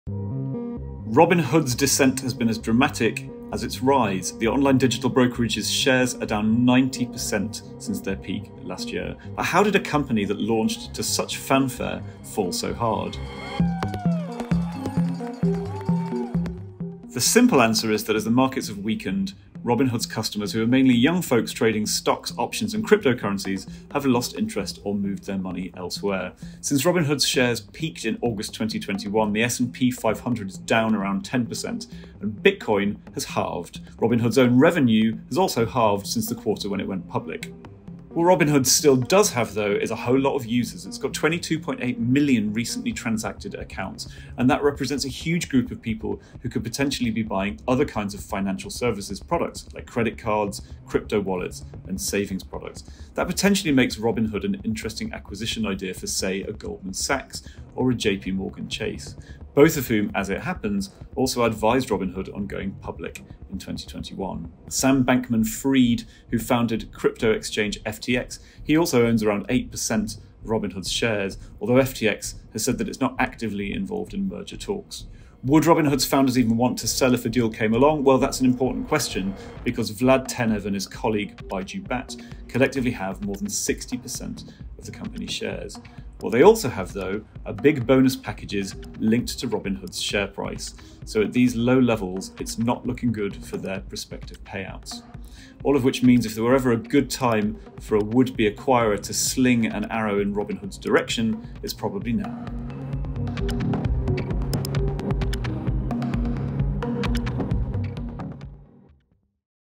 BVTV: Robinhood laid low
Robinhood’s descent has been as dramatic as its rise, but the company still has millions of users who are potential buyers of other financial products. Now could be a good time for a potential acquirer to step in, says John Foley.